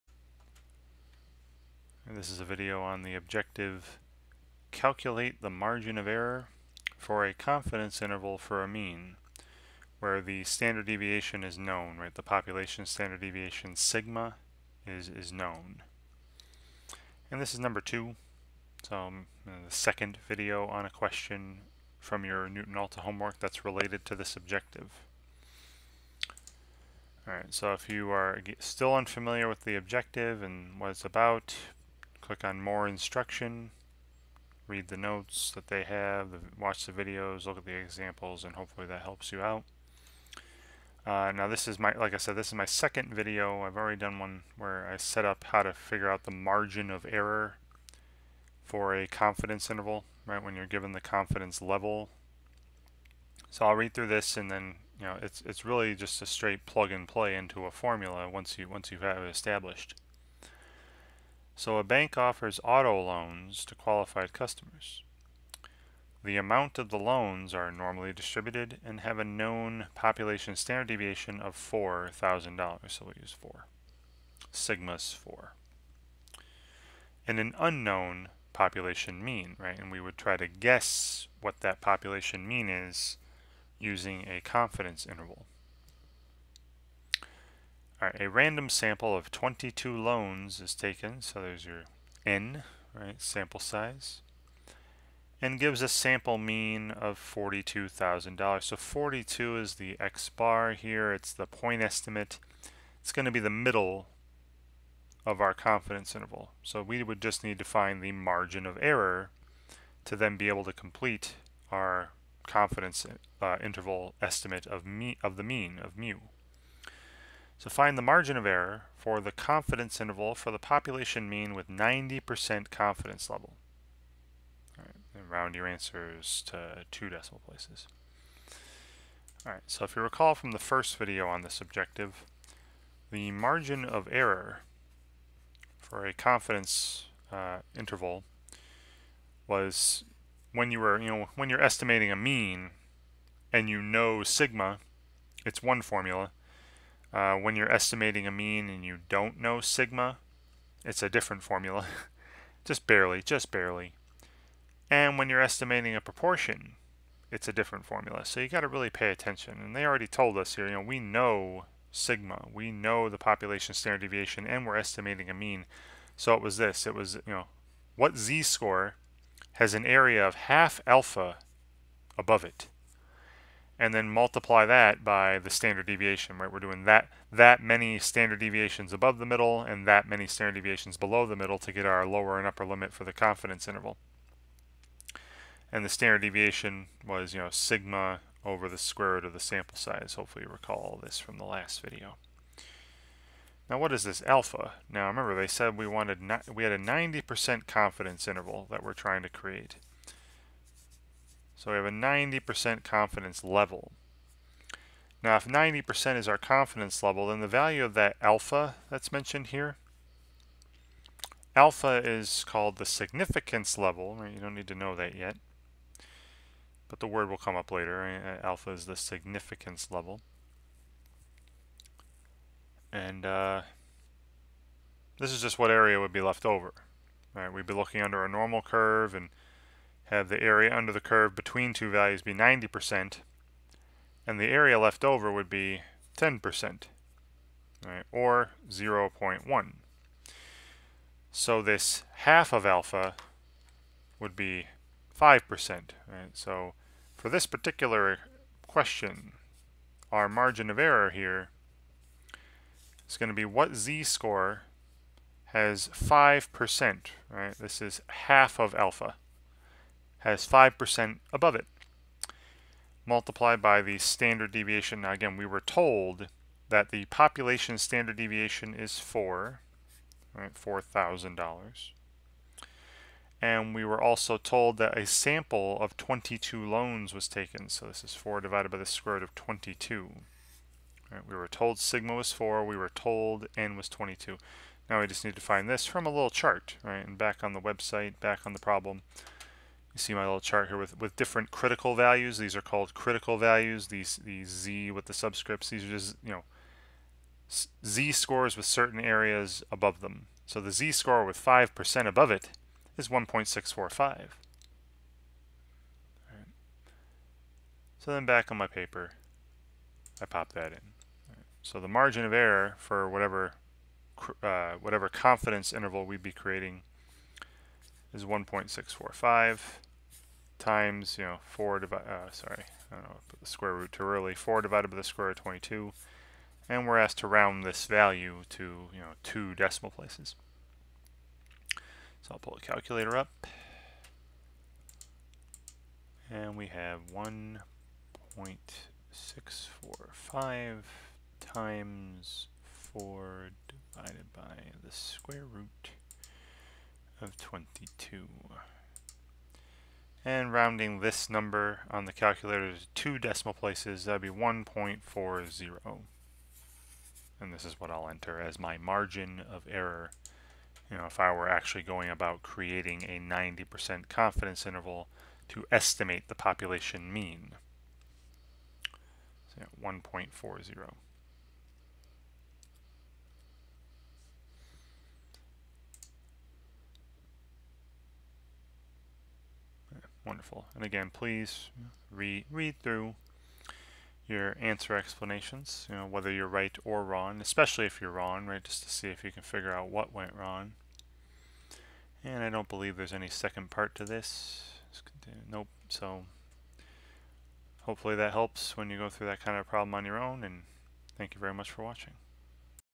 Calculate the margin of error for a confidence interval for a mean (standard deviation known) #2
This is the second video on a question related to Objective: Calculate the margin of error for a confidence interval for a mean (standard deviation known)